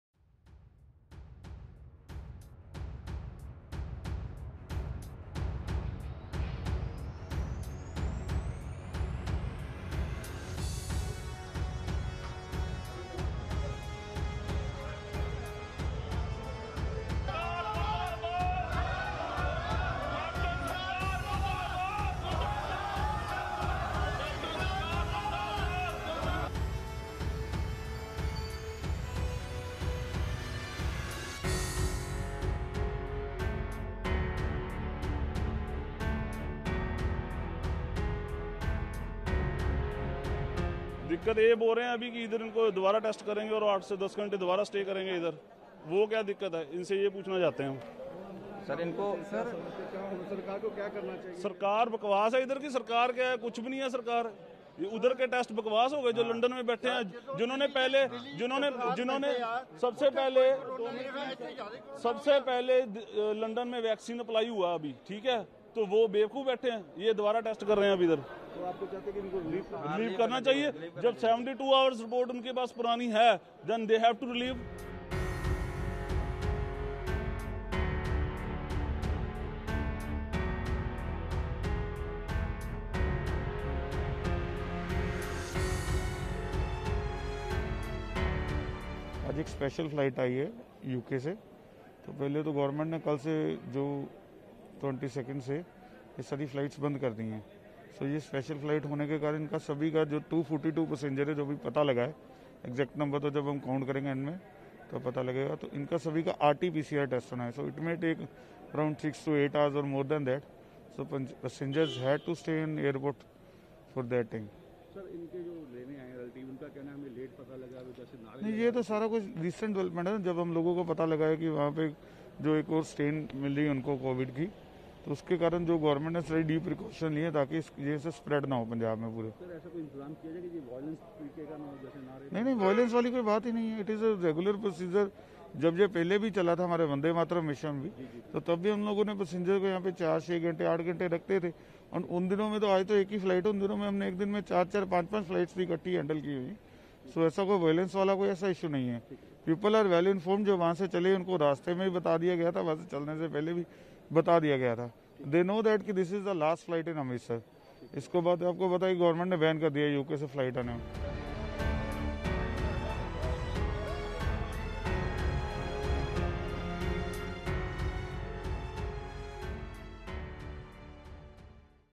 UK passengers undergo re-test on arrival at Amritsar airport, kin protest
Amritsar (Punjab), Dec 22 (ANI): Kin of passengers, who came from a flight from London, protested outside Amritsar airport on Dec 22. People waited outside airport for hours to receive their relatives. “Authorities are asking passengers to undergo COVID-19 testing even if they were tested just before boarding the flight. They should be allowed to go home,” a person said. “A total of 242 passengers have arrived on board this special flight. They need to undergo RT-PCT test which may take 6-8 hours and for that, passenger need to stay at the airport,” said Deepak Bhatia, SDM, Ajnala, Amritsar. A new strain of coronavirus has been found in UK. Yesterday, the government temporarily suspended all flights from UK till December 31.